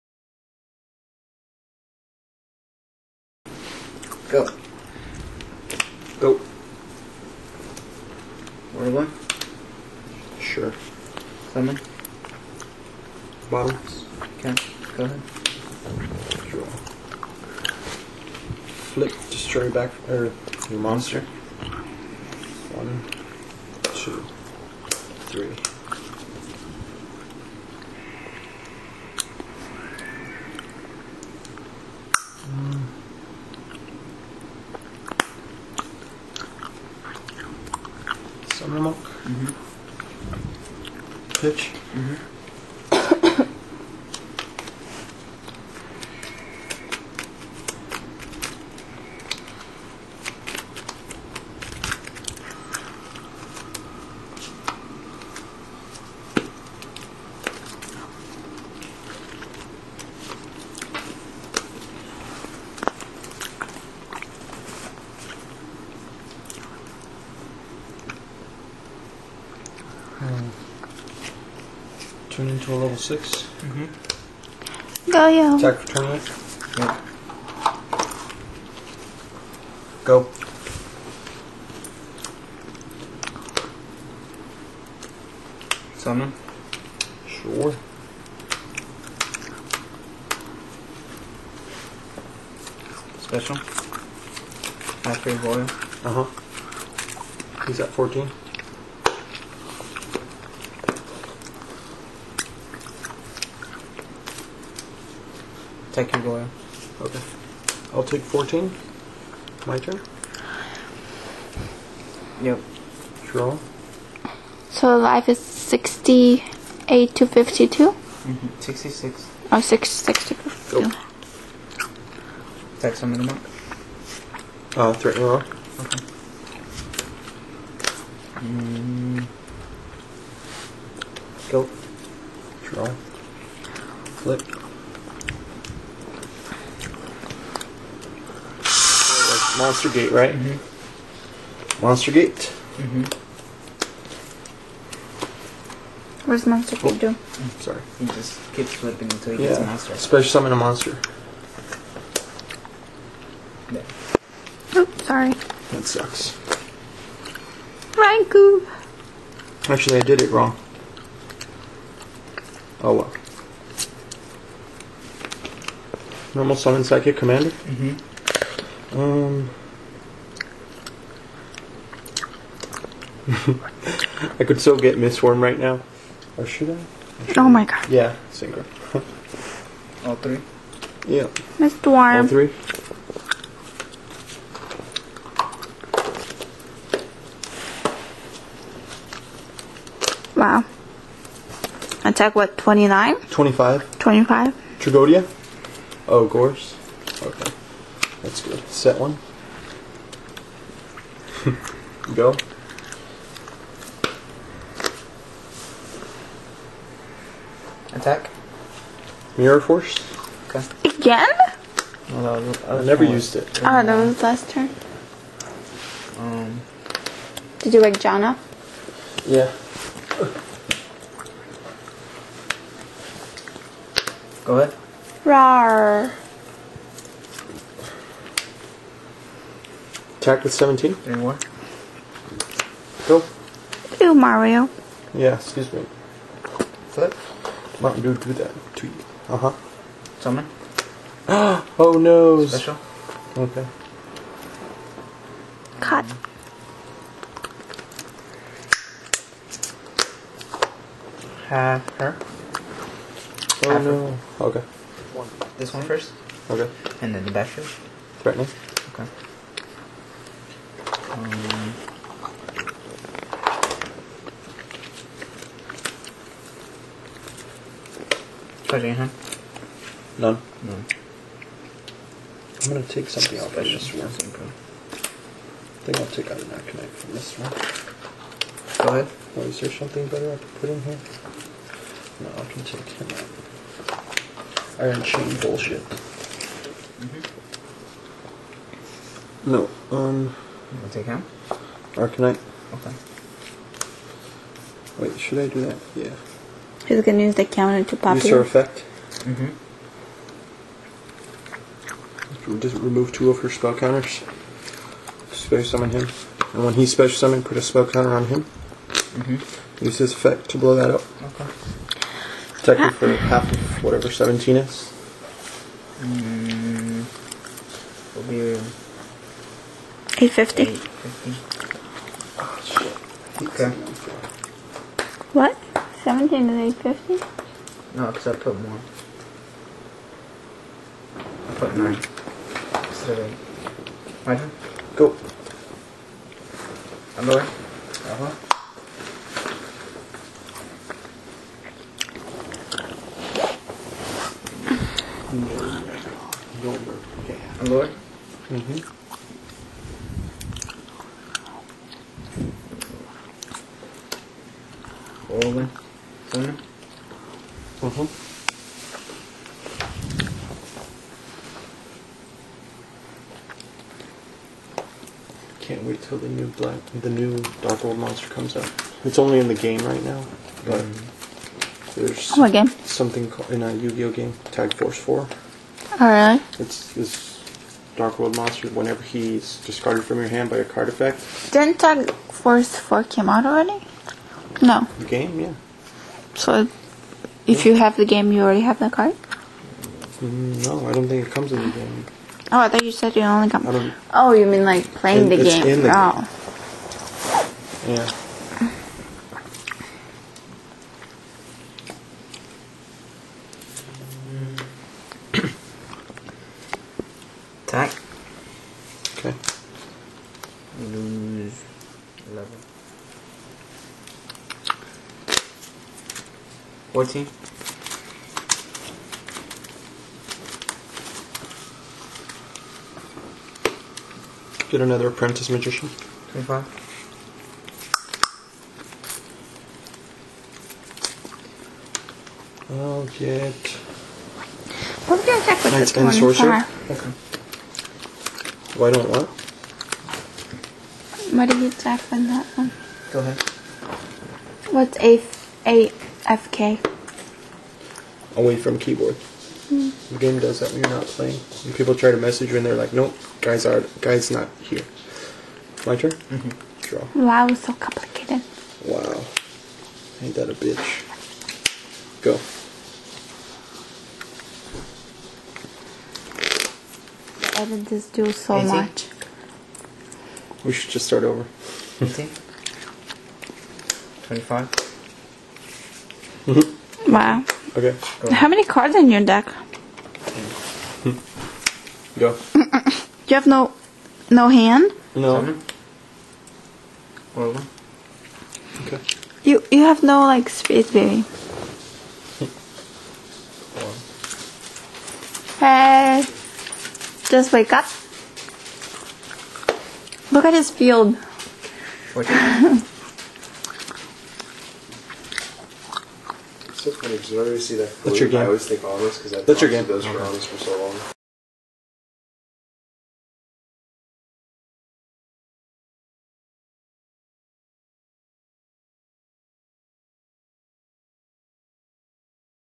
Flamewell Spellcasters Vs. Blackwings:)
Hey guys, sorry i havent been here for a while, buzy. but i will upload more stuff and a contest coming soon:) Heres a duel between Flamevell Spellcasers Vs Blackwings:) Sorry for the background noise:) ignore it lol:) Rate comment and subscribe:)